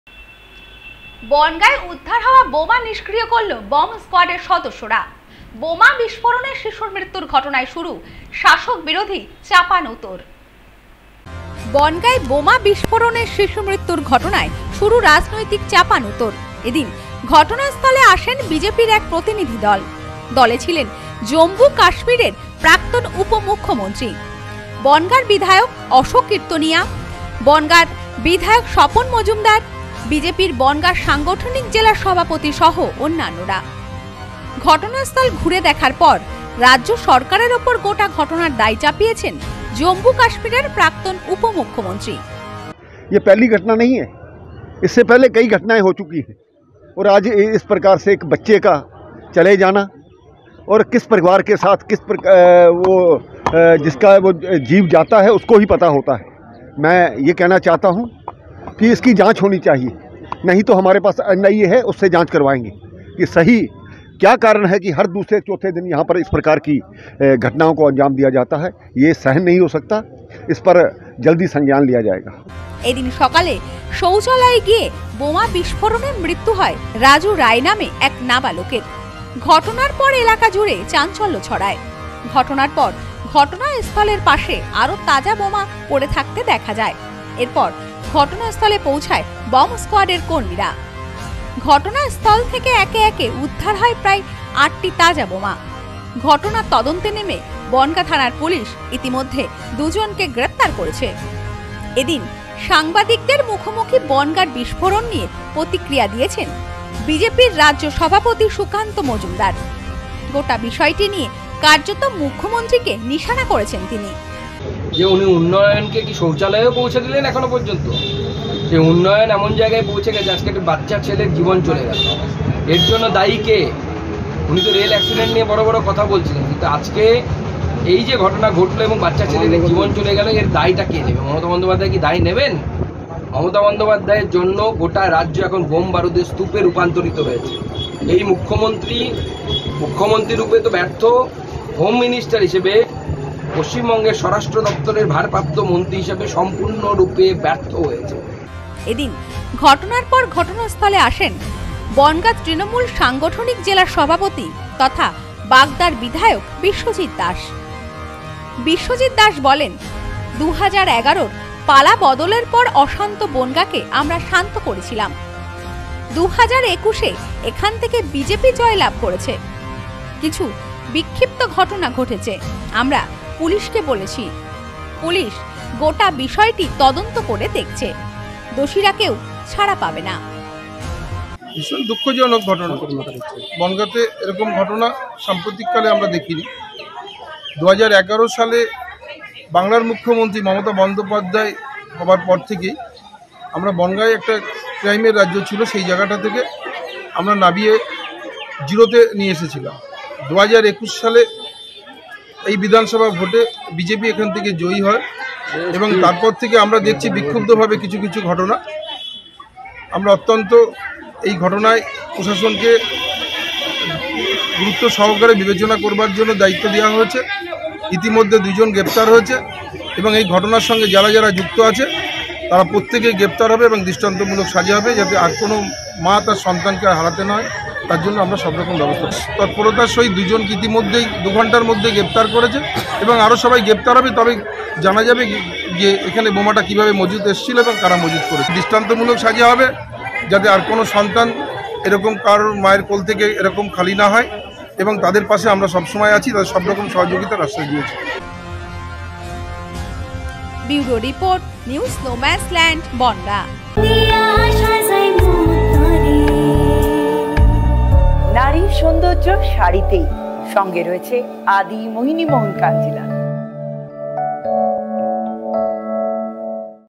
বনগাঁয় উদ্ধার হওয়া বোমা নিষ্ক্রিয় করল বম্ব স্কোয়ার্ড৷
বনগাঁয় উদ্ধার হওয়া বোমা নিষ্ক্রিয় করল বম্ব স্কোয়ার্ড৷ বোমা বিস্ফোরণে শিশু মৃত্যুর ঘটনায় শুরু শাষক বিরোধী চাপানউতর ৷